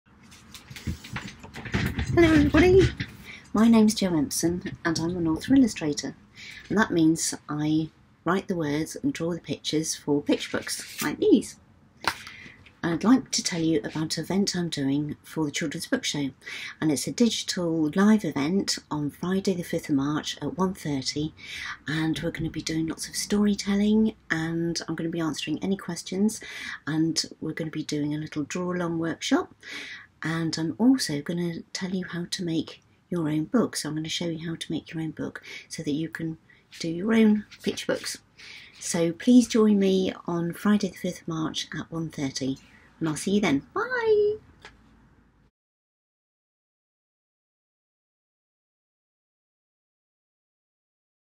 Jo Empson tell us about her digital performance
Author and illustrator Jo Empson tells us what she'll be doing during her live digital performance on Friday 5 March 2021